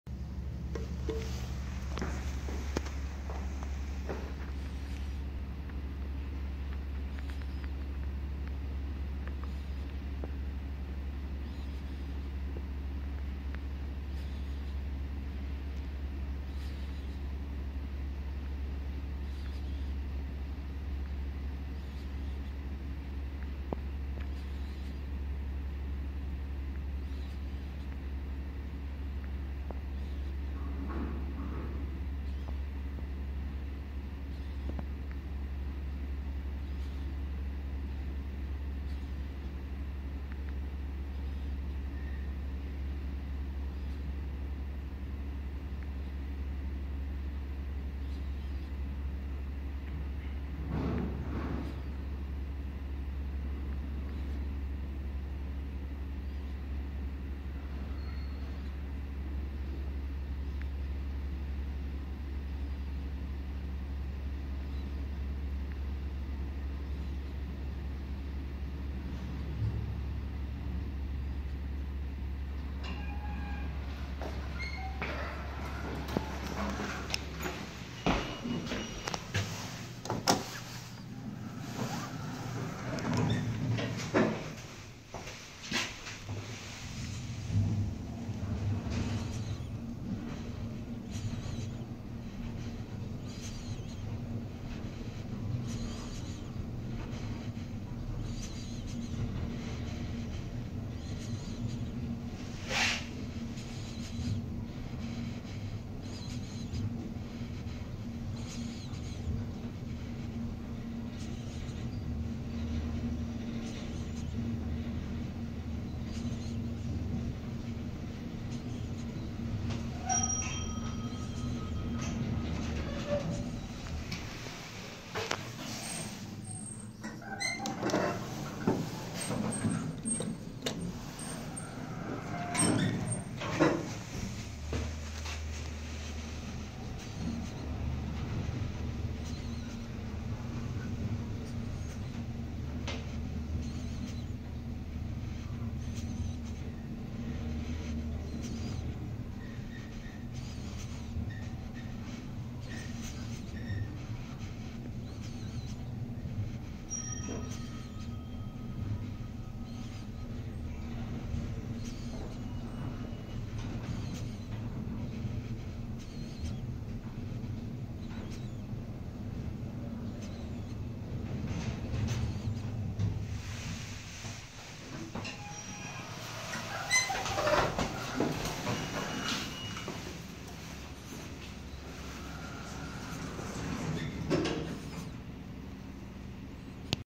Goldstar High Speed Traction Elevator at Hanshin Plaza, Seongnam Korea
Taken : 2019
Person : 17
Capacity : 1150kg
Install : 1992
Hall Lantern : O
Serviced Floor : B4~18
Model/Brand : HVP-I
Speed : 120M/MIN